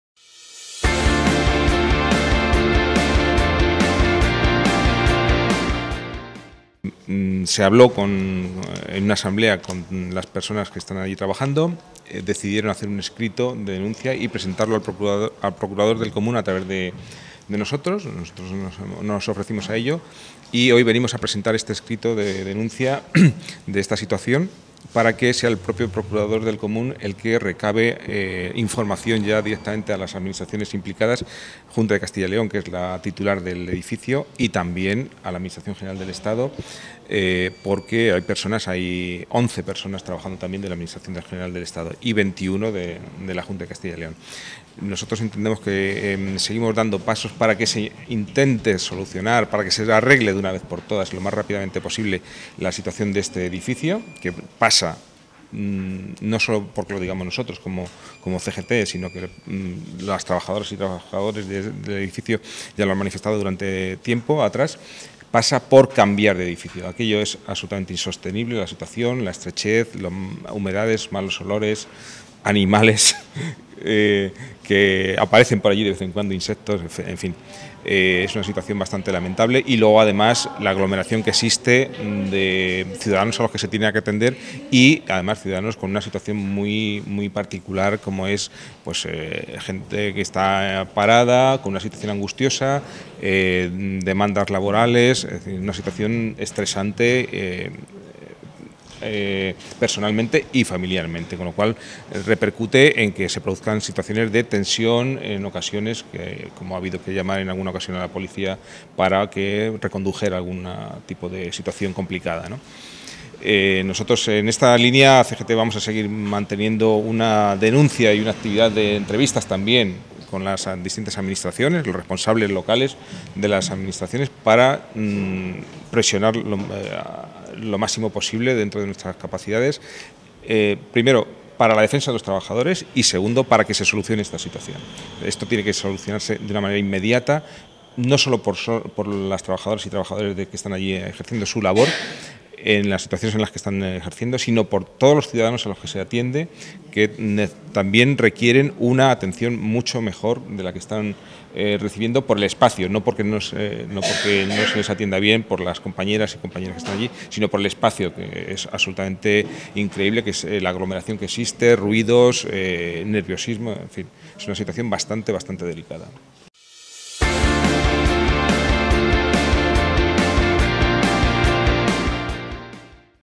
La situación de la Oficina de Empleo de Segovia, ante el Procurador del Común
Este martes, representantes de los trabajadores de la Oficina de Empleo han presentado ante el Procurador del Común un escrito de queja, junto con las firmas de los trabajadores, sobre la situación de las instalaciones de la Oficina de Empleo de Segovia, donde se ubican trabajadores tanto de la Junta de Castilla y León como del Servicio Público de Empleo Estatal. Denuncias que se vienen produciendo desde hace más de dos años. José Luis Lara, delegado de CGT en la Junta de Personal de la Junta de Castilla y León, ha declarado que "la solución pasa por cambiar de edificio".